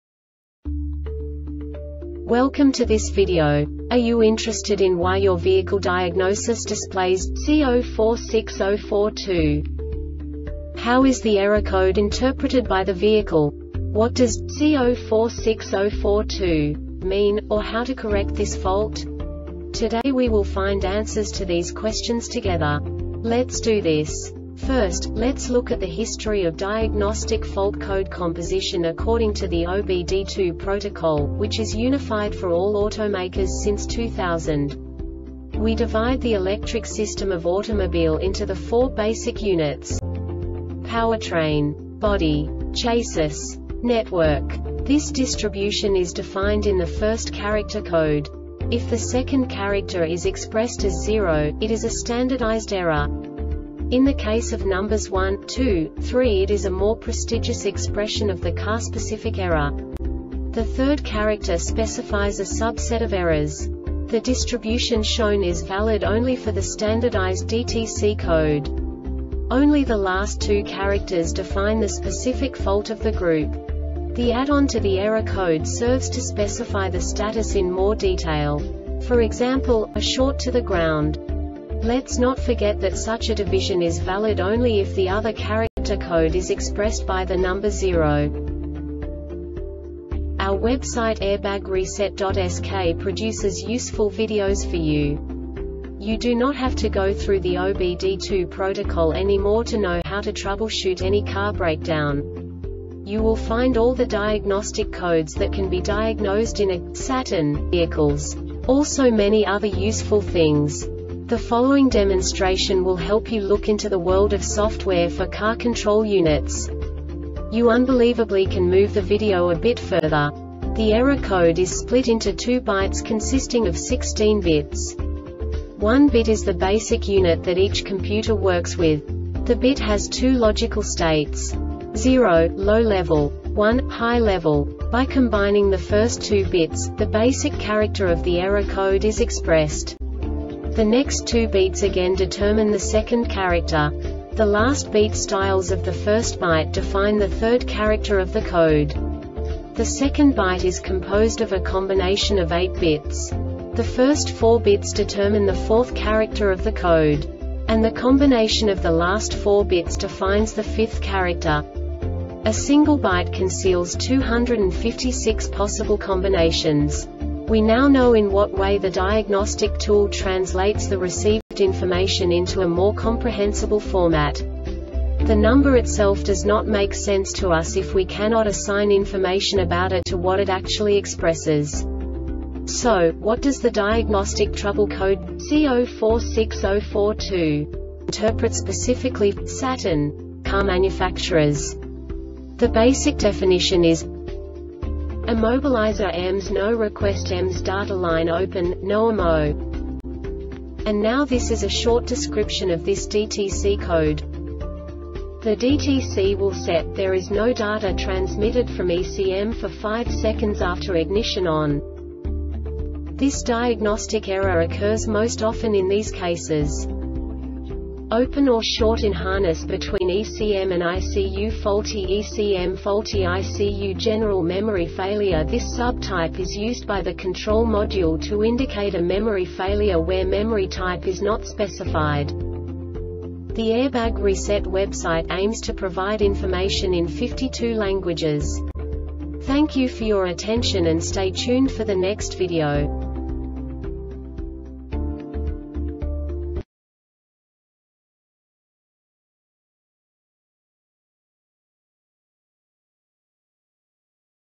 DTC Saturn C0460-42 Short Explanation
Contents:
0:21 Basic DTC analysis according to OBD2 protocol standard.
1:48 Insight into programming
2:58 A diagnostically understandable description of the Diagnostic Trouble Code
3:05 Basic error definition

Make:
Saturn
Code:
C0460 42
Definition:
Immobilizer-EMS No Request ( EMS data line open, no immo.)
Description:
The DTC will set,there is no data transmitted from ECM for 5 seconds after ignition ON.
Cause:
 Open or Short in harness between ECM and ICUFaulty ECMFaulty ICU
Failure Type:
General Memory Failure
This sub type is used by the control module to indicate a memory failure where memory type is not specified.